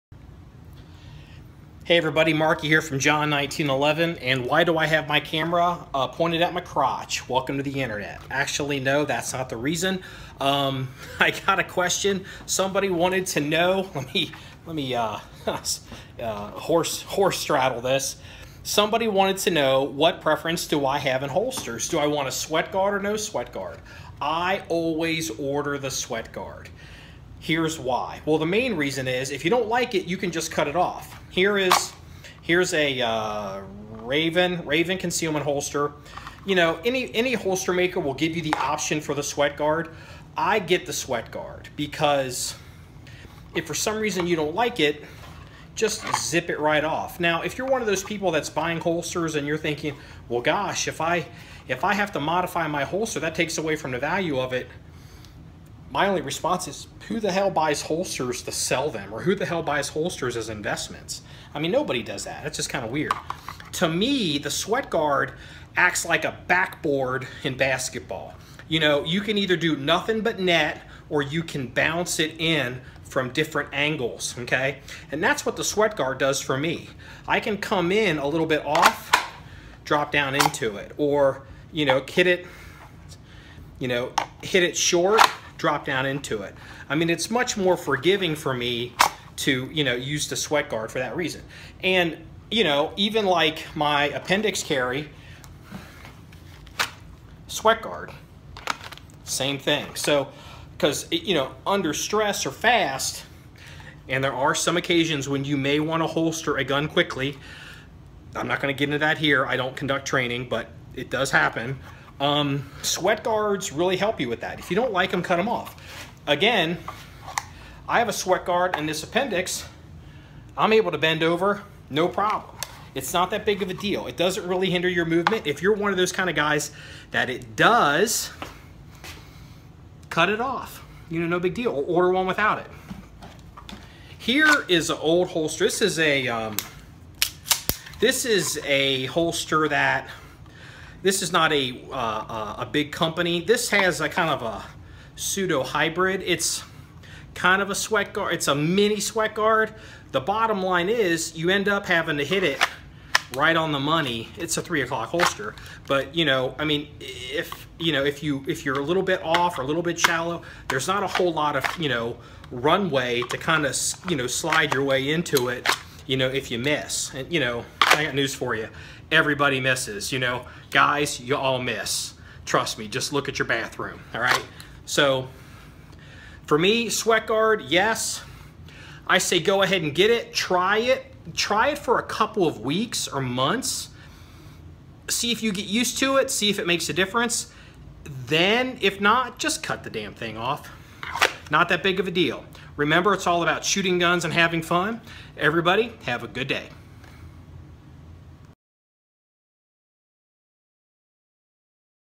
Holsters: Why I Always Choose Sweat Guards
Had a question from a viewer. He wanted to know what my position was on sweat guards for Kydex holster's?

I always get them.

Here's why.